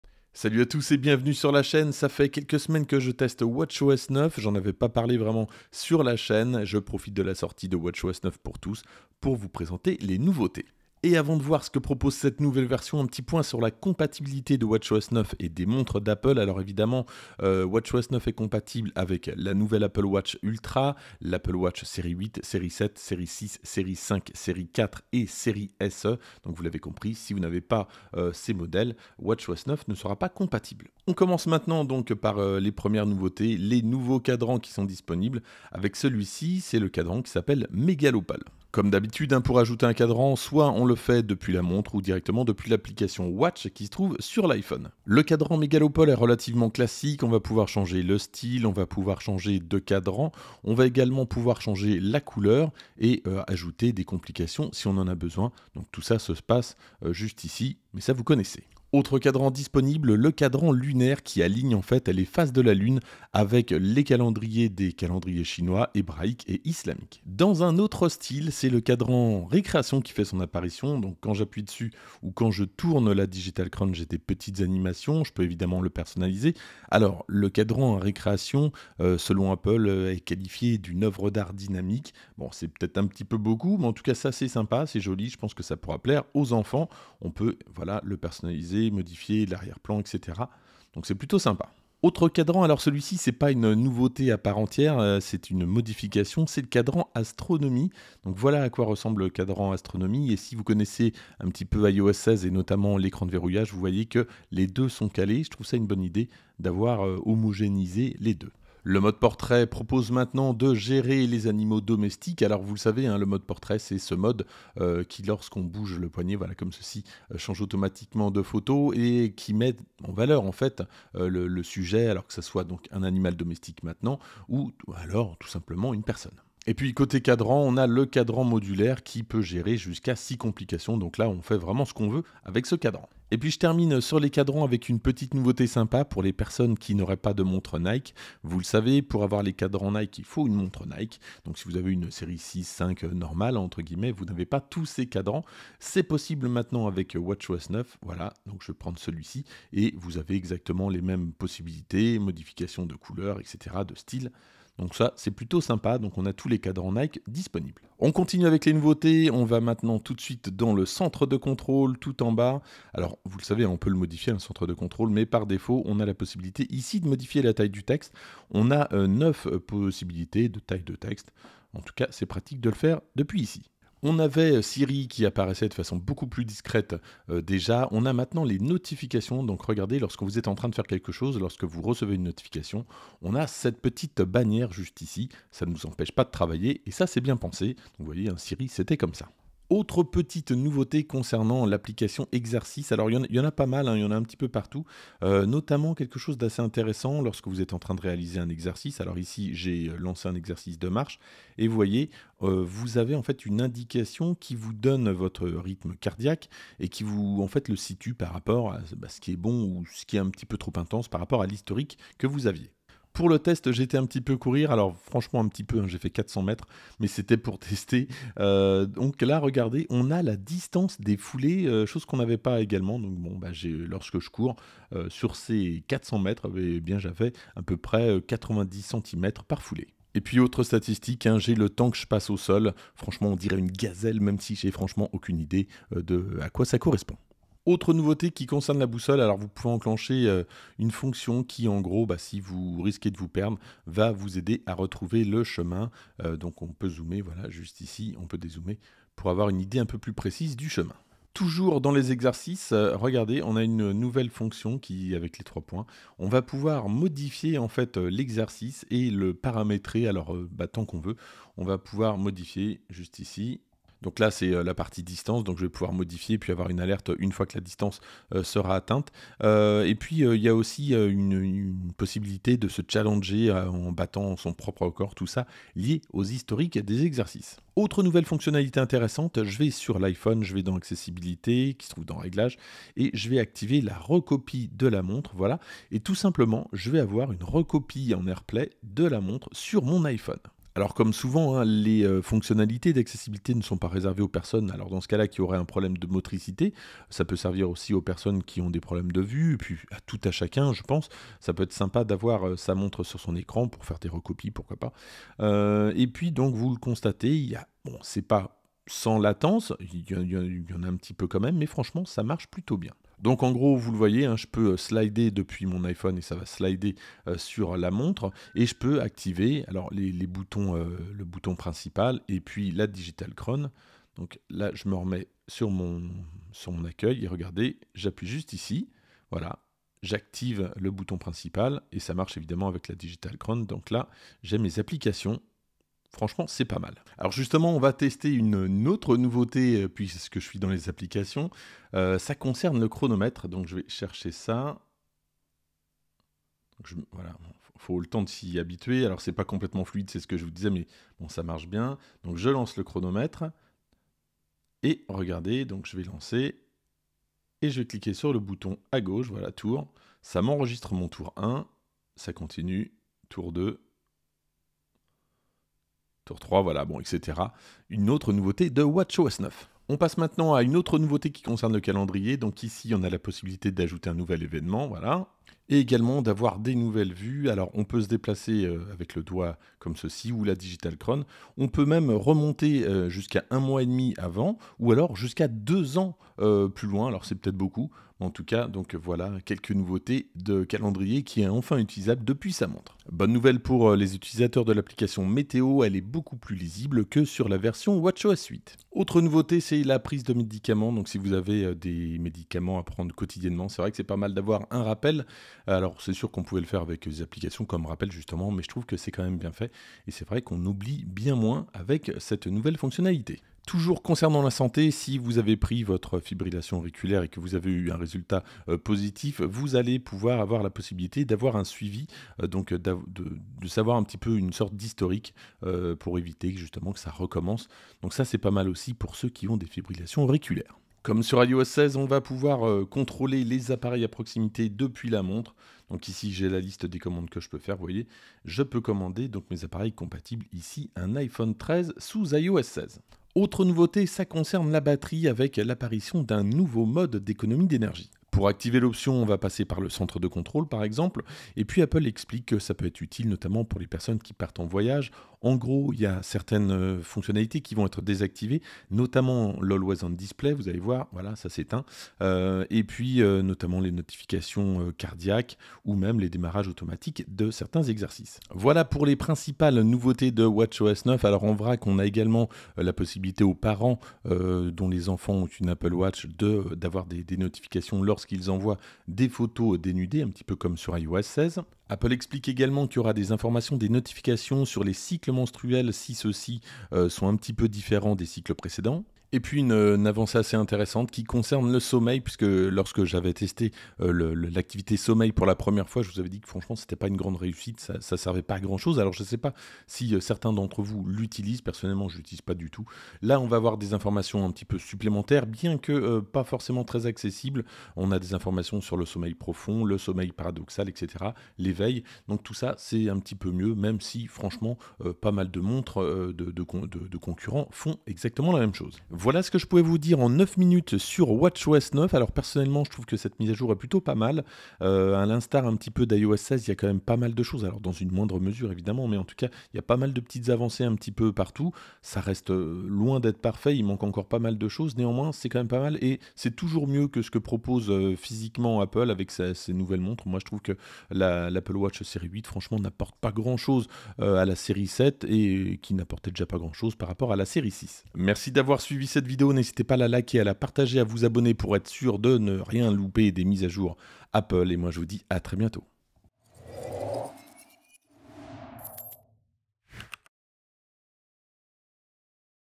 watchOS 9 : Nouveautés en 9 minutes de watchOS sur Apple watch (français)
Je vous propose de découvrir en 9 minutes (c'est original ! )  les nouveautés de watchOS 9 !
Achetez une Apple watch Series 8